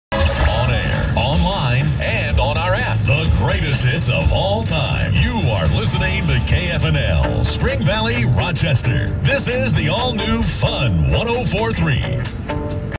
KFNL/Spring Valley, Minnesota Legal ID - December 13, 2020
Classic hits-formatted “Fun 104.3”, owned by Townsquare and serving the Rochester area.